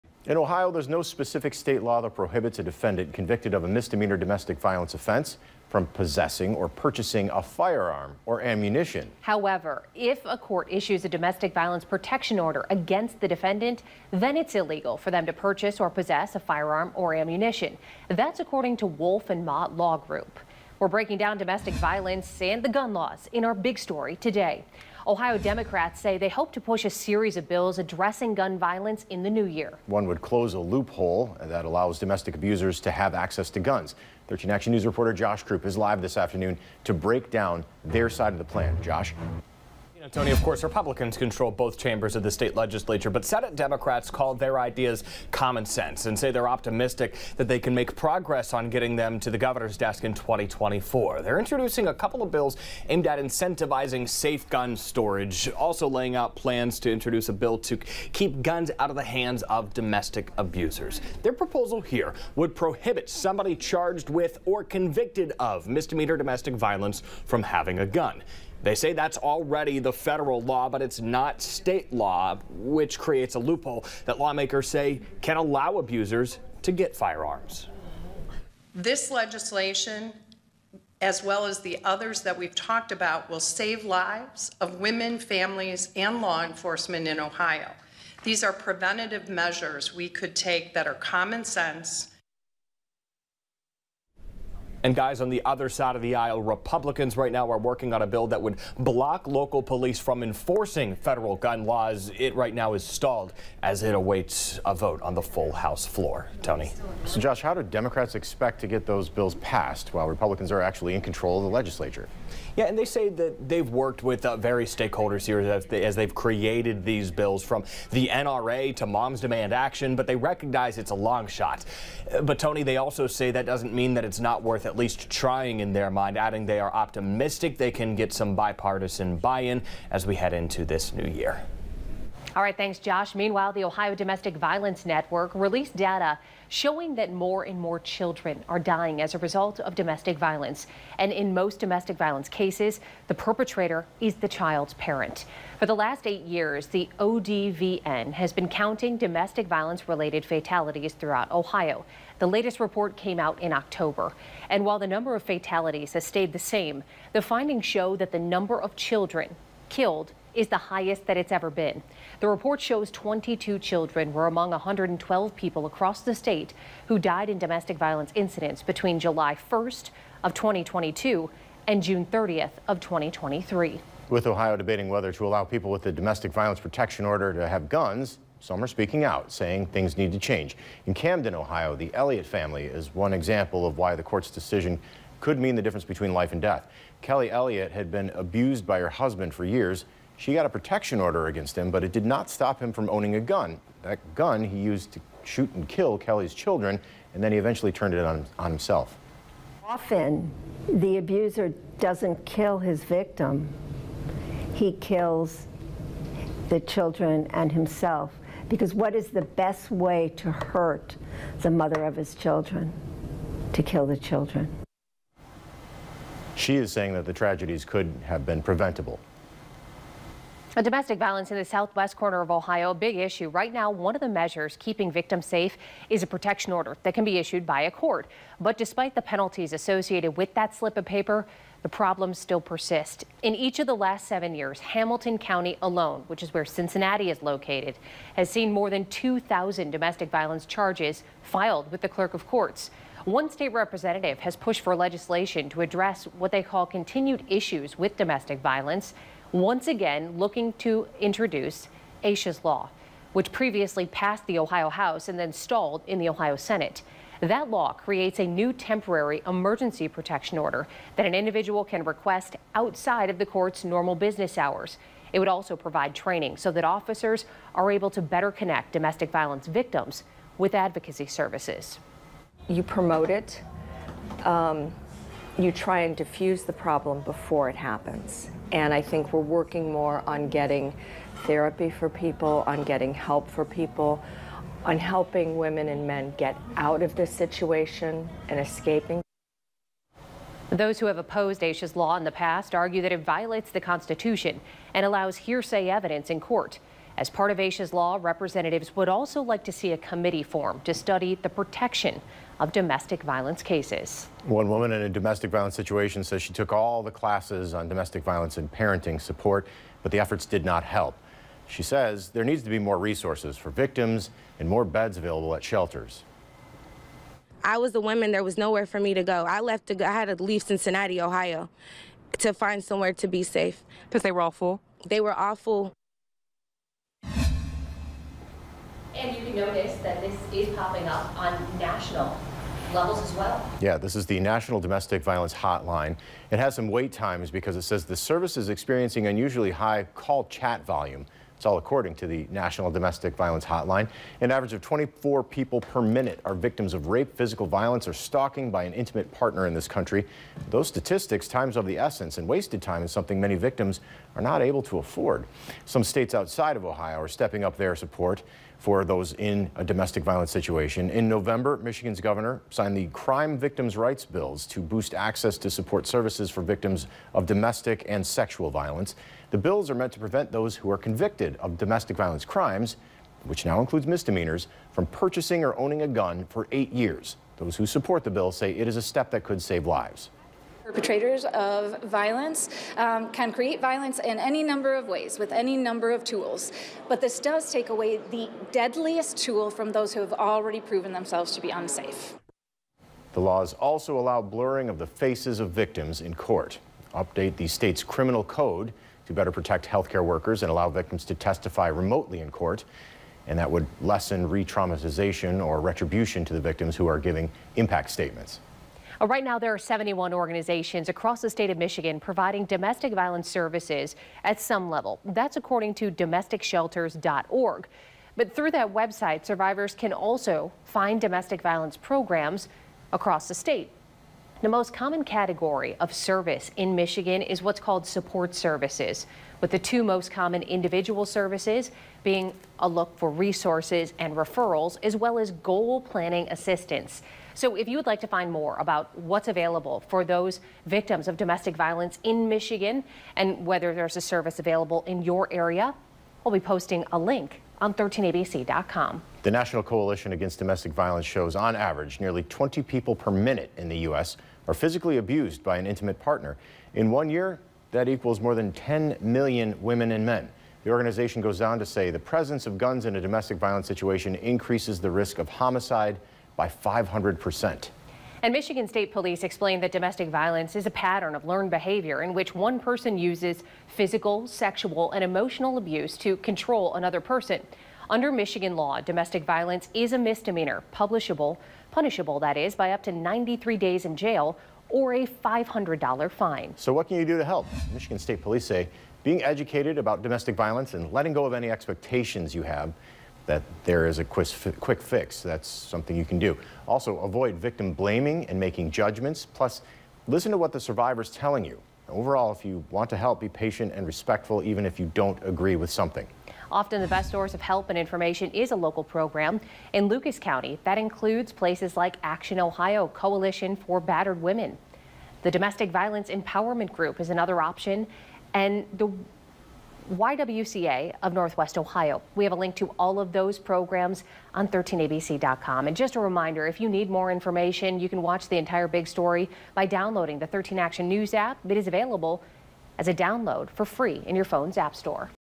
13 Action News Big Story: Domestic Violence and Gun Laws
In, Ohio, there is no law that prohibits a defendant convicted of a misdemeanor domestic violence offense from possessing a firearm or ammunition.